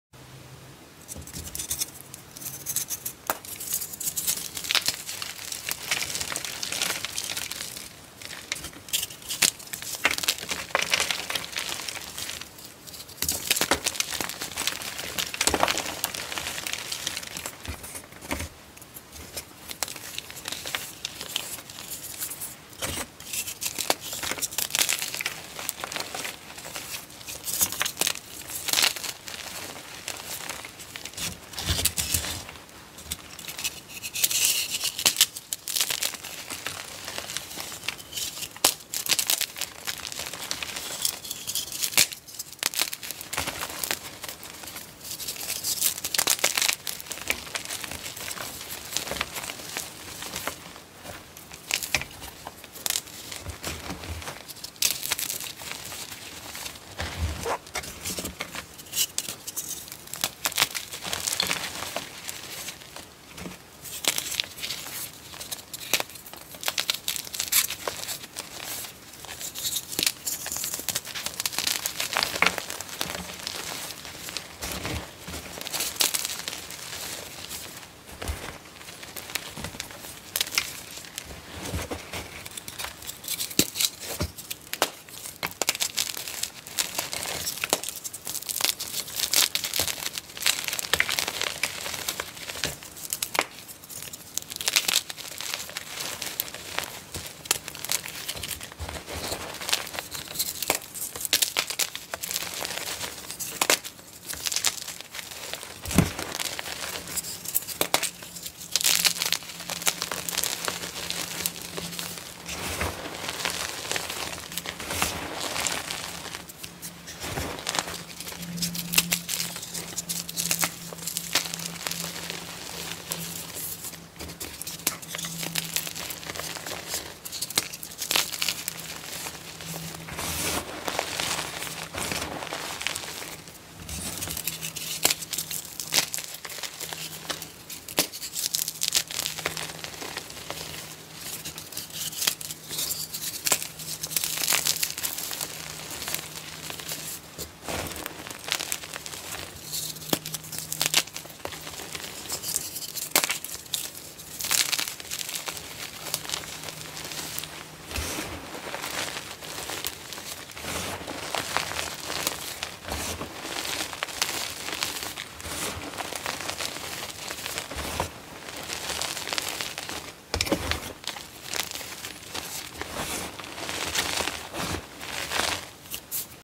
Happy Earth Day!! 🌎 ~ ASMR ~ Soft, Vibrant, BSN ~ Repost
I didn't have anything planned for Earth day so I thought a repost should be fine. This is one of my favorite videos. 😍 I hope you approve and enjoy!